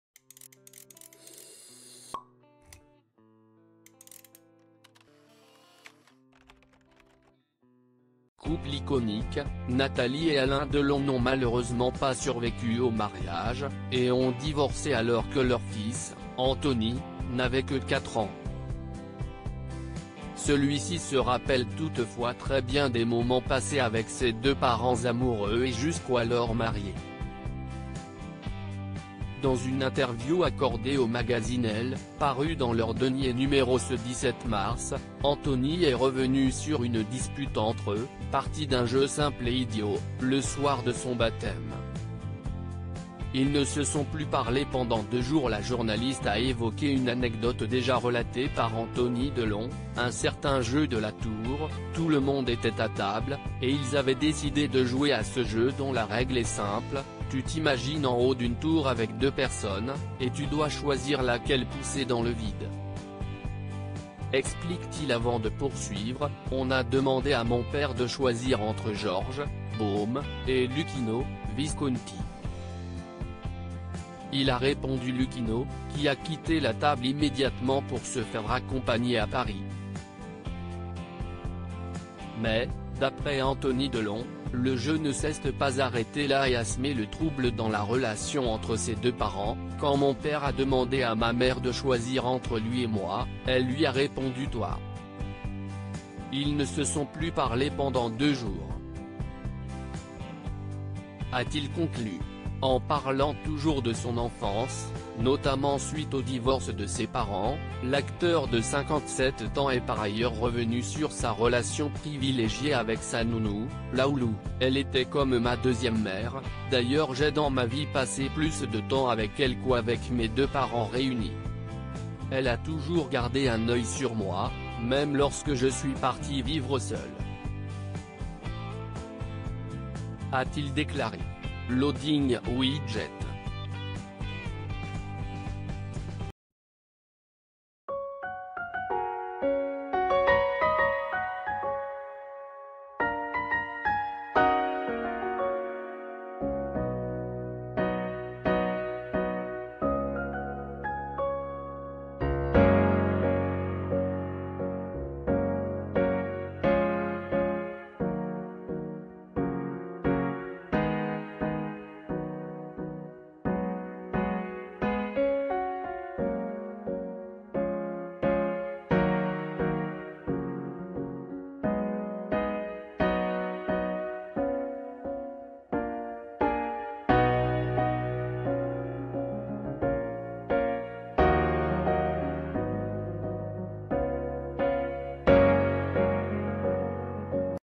“Ils ne se sont plus parlé” : ce jeu dérangeant en plein dîner qui a mis le feu aux poudres entre Al
“Ils ne se sont plus parlé” : ce jeu dérangeant en plein dîner qui a mis le feu aux poudres entre Alain et Nathalie Delon